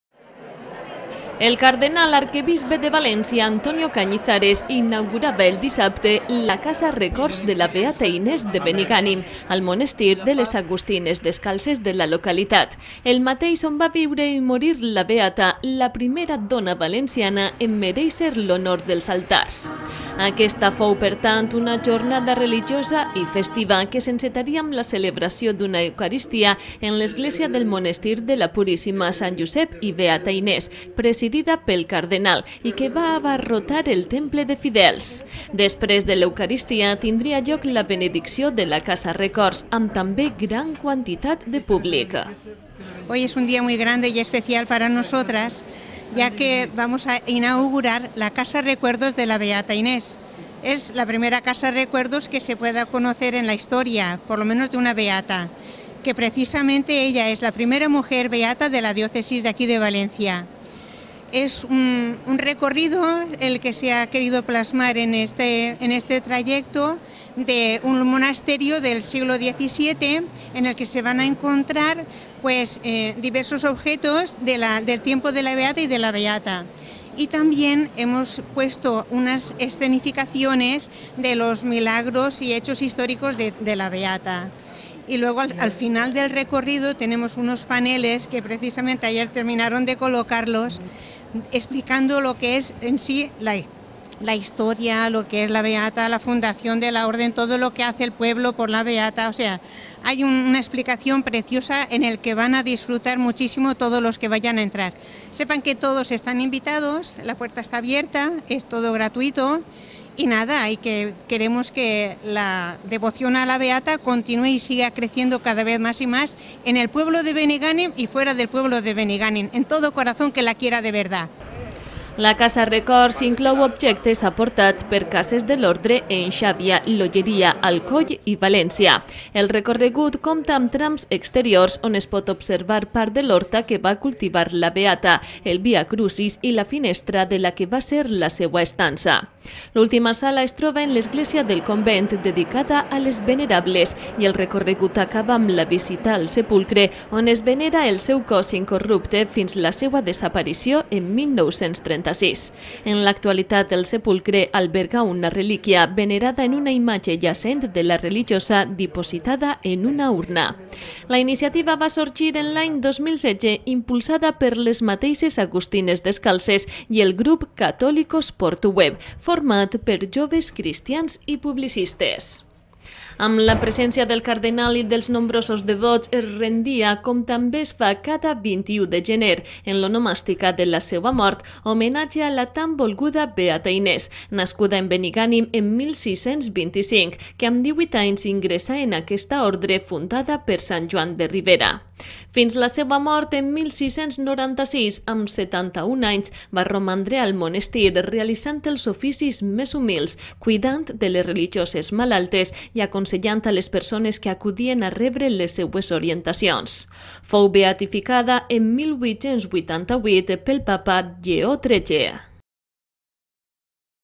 Inauguración
El 16 de septiembre quedó inaugurada la Casa Recuerdos de la Beata Inés. El 2º y 4º domingo de cada mes de 10,00 a 13,00 h abrimos la puerta  para los visitantes. Que la Beata Inés interceda por nosotros.